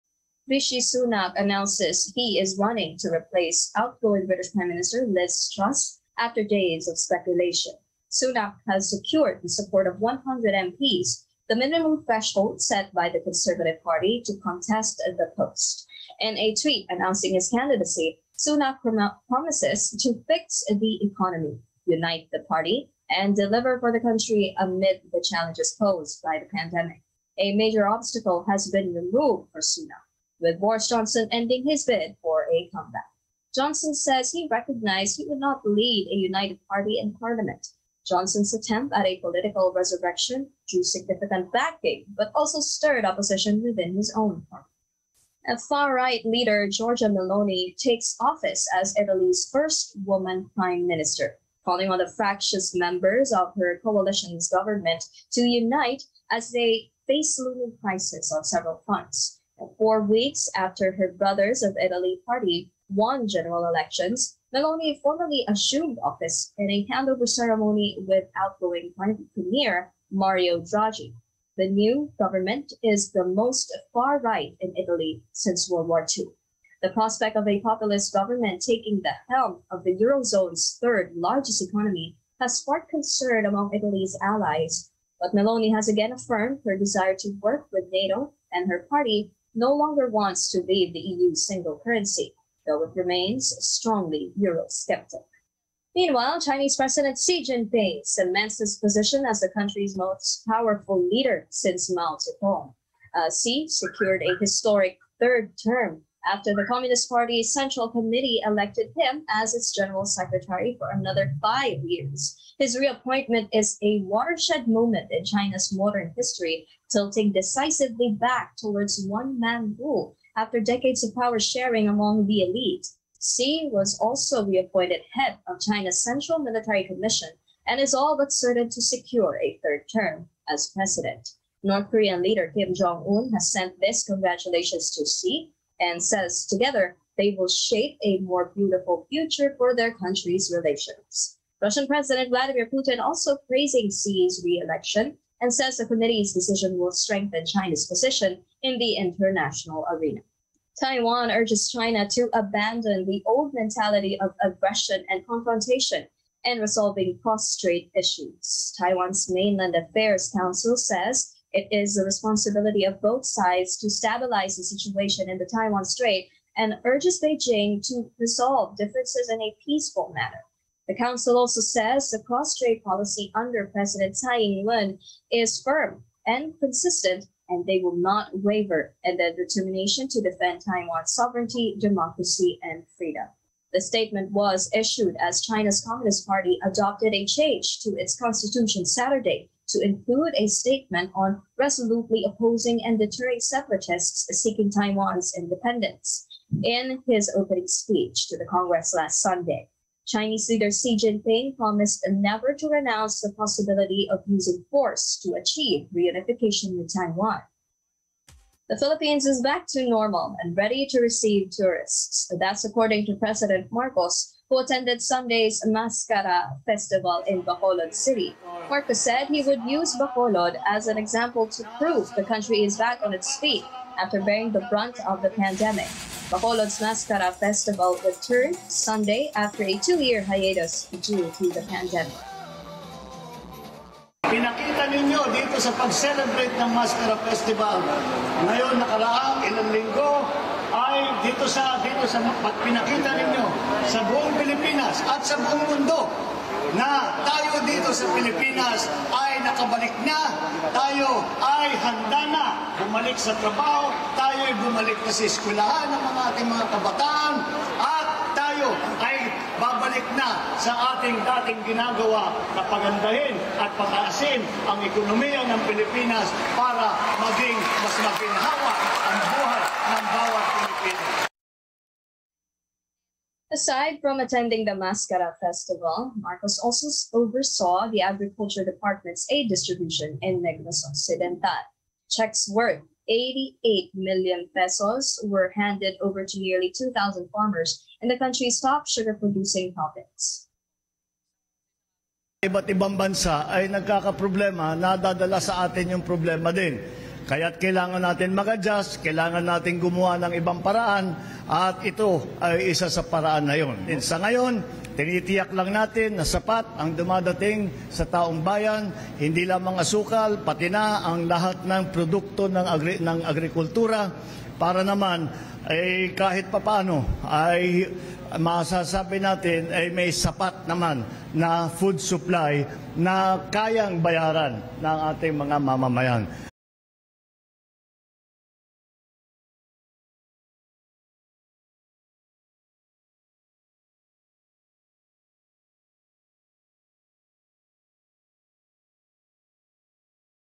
Boris Johnson drops out of UK Prime Minister race | ANC
Rundown: Rishi Sunak looked set to become Britain's next prime minister after Boris Johnson withdrew from the contest.